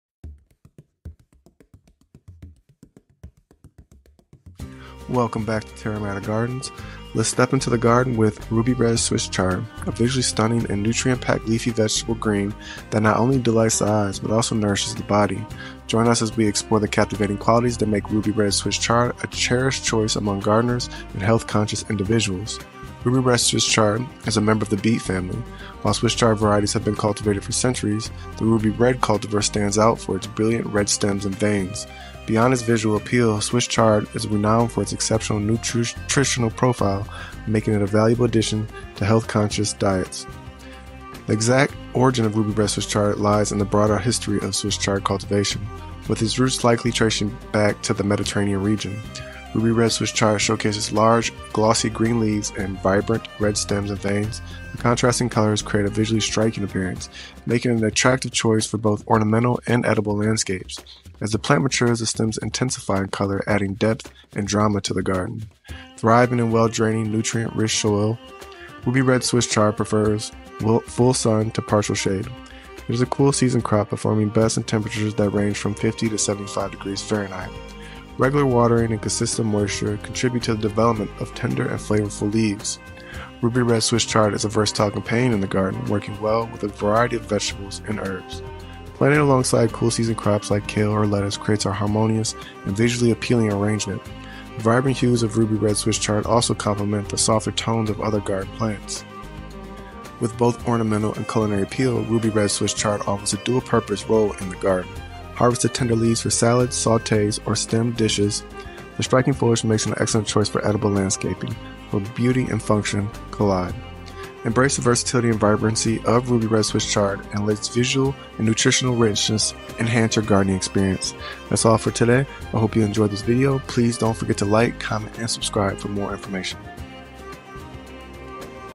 Exploring RUBY RED SWISS CHARD in 2 Minutes (Beta vulgaris)!
In today's video, we're embarking on an exciting journey to explore the world of RUBY RED SWISS CHARD. Join us as we dive into its rich history, learn about its unique characteristics, discover its preferred companions, and delve into valuable growing tips. We'll also explore its diverse uses in gardening and farming.

Here's a sneak peek at what we'll cover:

Latin name: Beta vulgaris subsp. cicla
Common name: Ruby Red Swiss Chard
Type: Leafy green, biennial
Sun Exposure: Full sun to partial shade
Soil Requirements: Well-draining, fertile soil
Watering: Keep soil consistently moist; regular watering
Height: Typically 18 to 24 inches
Spacing: Plant 12 to 18 inches apart
Leaf Color: Deep red to burgundy
Stem Color: Bright red
Harvesting: Leaves can be harvested when young for salads, and mature leaves can be cooked
Days to Germination: 7 to 14 days
Days to Maturity: 50 to 60 days from germination
Frost Tolerance: Cold-hardy; can withstand light frost
Hardiness Zones: Swiss chard is usually grown as an annual and is suitable for zones 3 to 10
Special Features: Edible leaves and stems, ornamental in vegetable gardens
Uses: Culinary use in salads, sautés, and other cooked dishes
Maintenance: Remove any damaged or yellowing leaves regularly
Growing Tips: Swiss chard is relatively easy to grow and can tolerate a range of conditions; provide consistent moisture for best results
Pest and Disease Resistance: Generally resistant to pests and diseases; watch for leaf miners and aphids
Note: Ruby Red Swiss Chard is a vibrant and nutritious leafy green with attractive red stems and veins. It is not only a culinary delight but also adds visual interest to vegetable gardens and ornamental plantings.